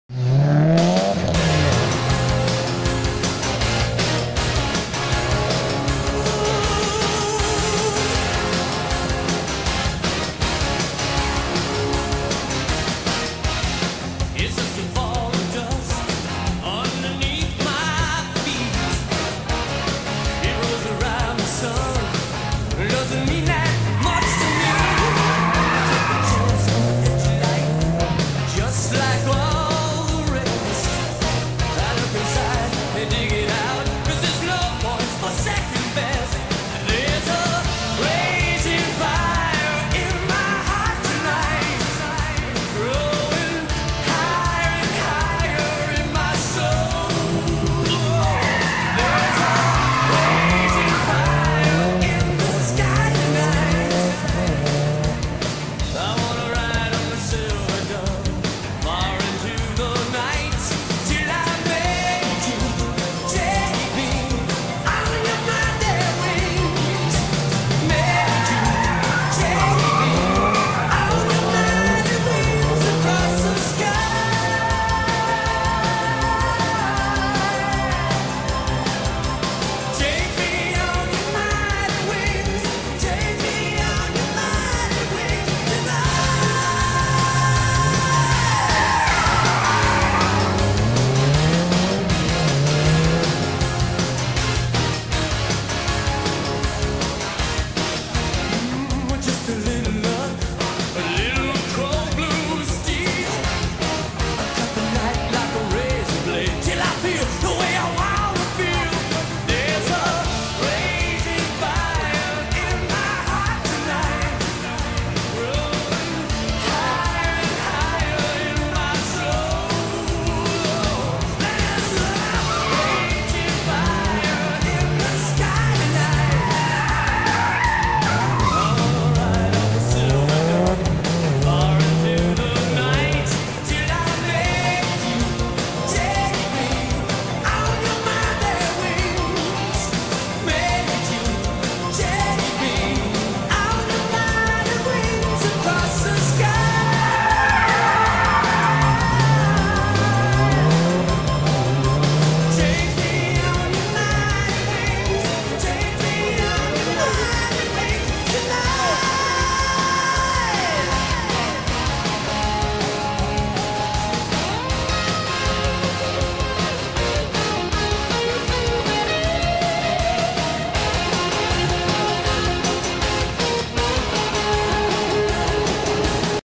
Subaru slalom Top Gun 27/05/2018
Raduno Fiat Abarth Club Sicilia 27/05/2018
Staff e apri pista Subaru Impreza STI by Subaru Sicilia